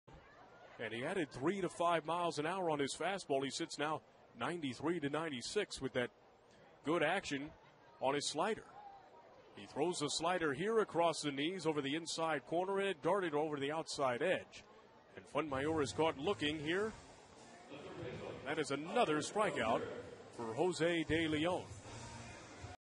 Tulsa's De Leon gets eighth strikeout
5/22/15: Tulsa's Jose De Leon notches the eighth strikeout of his Double-A debut for Tulsa in a loss at NW Arkansas